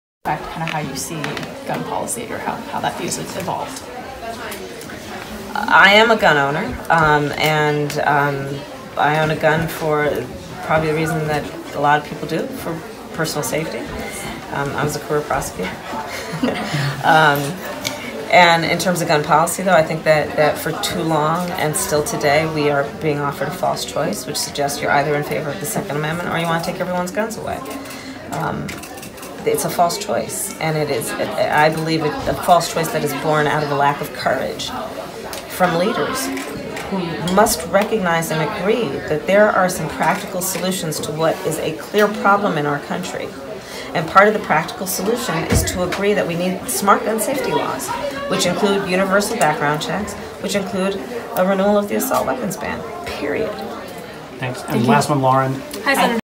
Double Standards DoubleTalk Doublethink Democrat Pamela Harris
When sane adults with clean criminal records decide they need an "assault weapon," a 100 miles from law enforcement, it should not be up to the government do decide whether you have a 10 round or 30 round magazine to protect you from home invasion. Self-defense is a self-evident, as well as a natural inalienable right. It is not a privilege anymore than the First Amendment is.